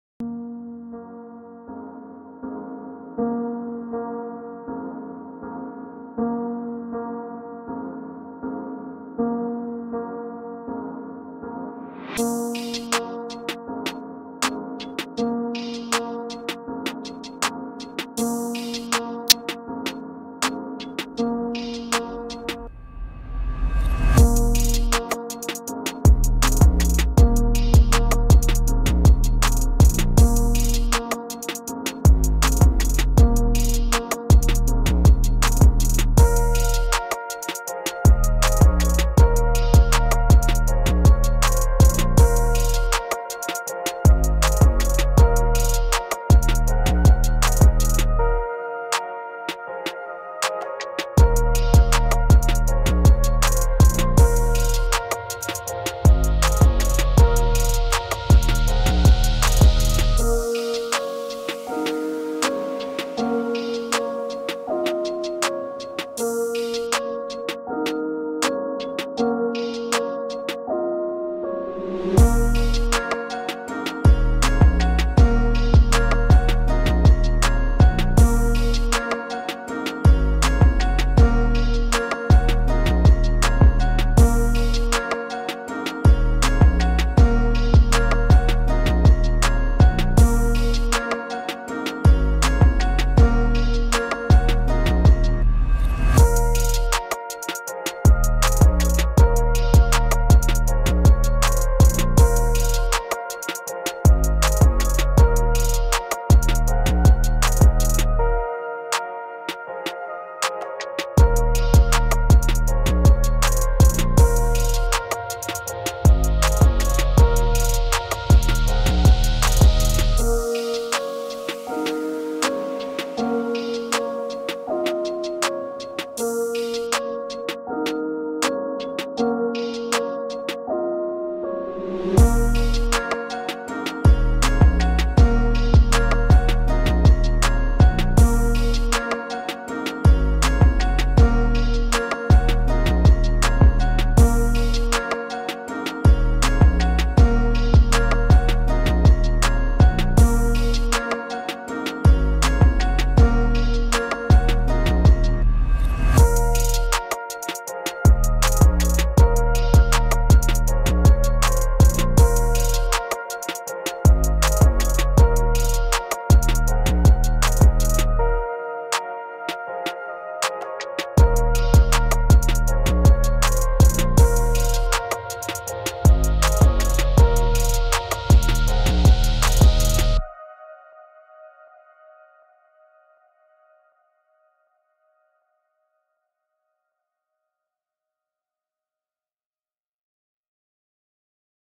Dark 808 Trap Beat | Luciano x Rin type Hip-Hop Instrumental | by Ritaro Beats
FREE] Dark 808 Trap Beat | Luciano x Rin type Hip-Hop Instrumental

♬ Production By Ritaro

This beat is free for non commercial use and has no copyright, give credit when you use it.